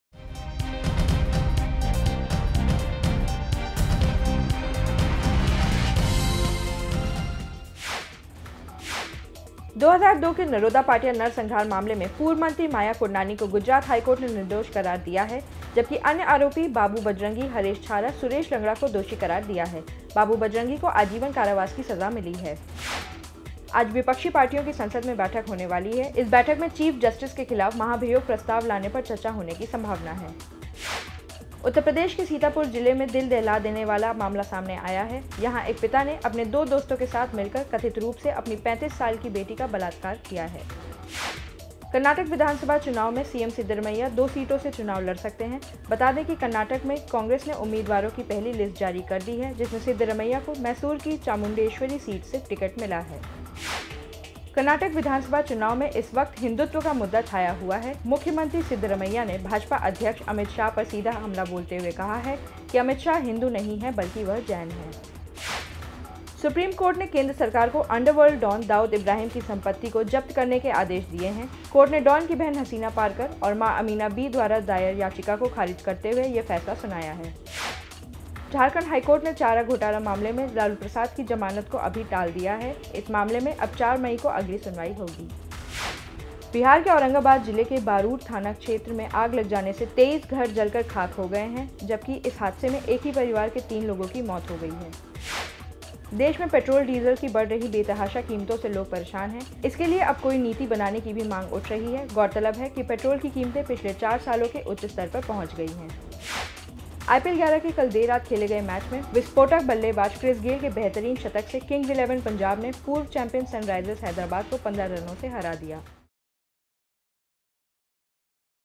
इस वक़्त की बड़ी खबरें - 20 April 2018
1 2002 के नरोदा पाटिया नरसंहार मामले में पूर्व मंत्री माया कोडनानी को गुजरात हाईकोर्ट ने निर्दोष करार दिया है. जबकि अन्य आरोपी बाबू बजरंगी, हरेश छारा, सुरेश लंगड़ा को दोषी करार दिया गया है. बाबू बजरंगी को आजीवन कारावास की सजा मिली है.

2 आज विपक्षी पार्टियों की संसद में बैठक होने वाली है. इस बैठक में चीफ जस्टिस के खिलाफ महाभियोग प्रस्ताव लाने पर चर्चा होने की संभावना है...

3 उत्तर प्रदेश के सीतापुर जिले में दिल दहला देने वाला मामला सामने आया है. यहां एक पिता ने अपने दो दोस्तों के साथ मिलकर कथित रूप से अपनी 35 साल की बेटी का बलात्कार किया है.

4 कर्नाटक विधानसभा चुनाव में सीएम सिद्धारमैया दो सीटों से चुनाव लड़ सकते हैं. बता दें कि कर्नाटक में कांग्रेस ने उम्मीदवारों की पहली लिस्ट जारी कर दी है जिसमें सिद्धारमैया को मैसूर की चांमुडेश्वरी सीट से टिकट मिला है...

5 कर्नाटक विधानसभा चुनाव में इस वक्त हिंदुत्व का मुद्दा छाया हुआ है. मुख्यमंत्री सिद्धारमैया ने भाजपा अध्यक्ष अमित शाह पर सीधा हमला बोलते हुए कहा है कि अमित शाह हिंदू नहीं है बल्कि वह जैन हैं.

6 सुप्रीम कोर्ट ने केंद्र सरकार को अंडरवर्ल्ड डॉन दाऊद इब्राहिम की संपत्ति को जब्त करने के आदेश दिए हैं. कोर्ट ने डॉन की बहन हसीना पारकर और मां अमीना बी द्वारा दायर याचिका को खारिज करते हुए यह फ़ैसला सुनाया है.

7 झारखंड हाई कोर्ट ने चारा घोटाला मामले में लालू प्रसाद यादव की जमानत को अभी टाल दिया है। इस मामले में अब चार मई को अगली सुनवाई होगी।

8 बिहार के औरंगाबाद जिले के बारूण थाना क्षेत्र में आग लग जाने से 23 घर जलकर खाक हो गए जबकि इस हादसे में एक ही परिवार के तीन लोगों की मौत हो गई.

9 देश में पेट्रोल-डीजल की बढ़ रही कीमतें बेतहाशा कीमतों से लोग परेशान हैं. इसके लिए अब कोई नीति बनाने की भी मांग उठ रही है...गौरतलब है कि पेट्रोल की कीमतें पिछले चार साल के उच्च स्तर पर पहुंच गई हैं.

10 आईपीएल 11 के कल देर रात खेले गए मैच में विस्फोटक बल्लेबाज क्रिस गेल के बेहतरीन शतक से किंग्स इलेवन पंजाब ने पूर्व चैंपियन सनराइजर्स हैदराबाद को 15 रनों से हरा दिया.